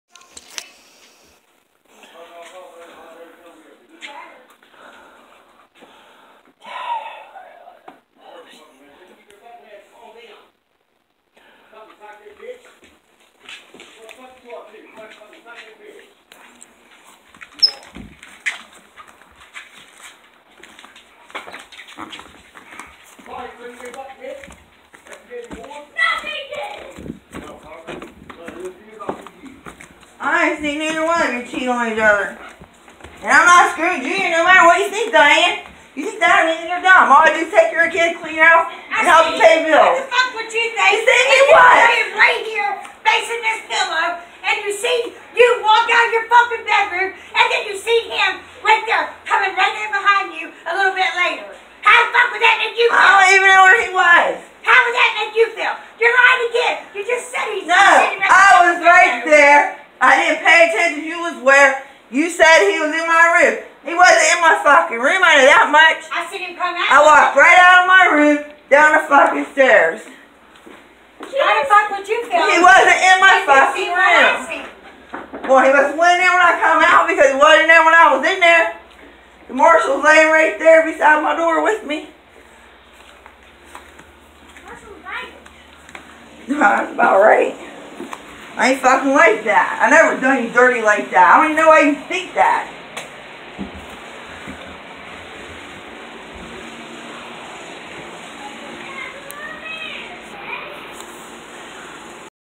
Listen to this stuff ..my so called friend
Trying blame me sex with her man..IM NO DIRTY HO LIKE THAT .